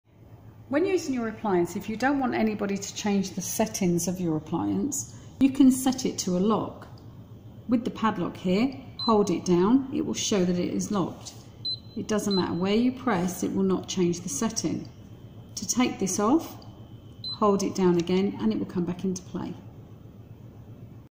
AEG / Electrolux / Zanussi - Electric and induction hob - Child lock 'L' code - V1 0
Where there is an 'L' displayed - what it means, how to activate and deactivate it.  This is known as the 'Child lock' - by using this, it ensures that none of the zones can be changed.